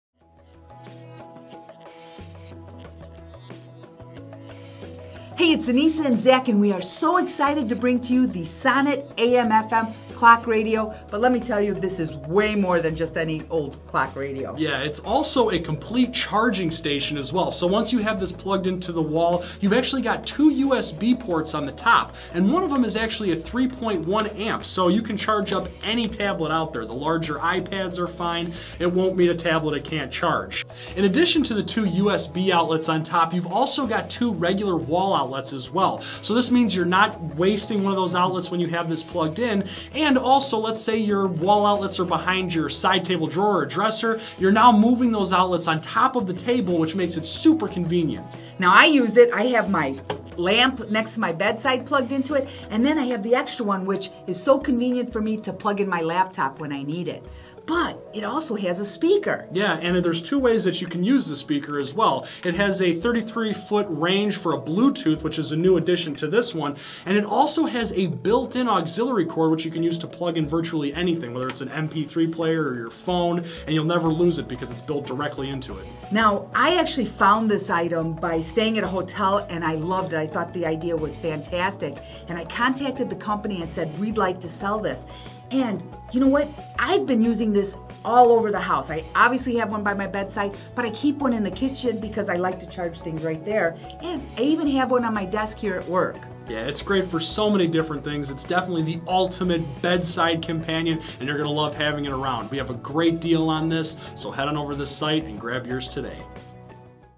Sonnet Clock Radio with Bluetooth Speaker and Charging Station
Here's the perfect night time companion... the Sonnet AM/FM Clock Radio is so much more than this. It actually has two USB Charging Ports in it. One port even powerful enough to charge a tablet or iPad!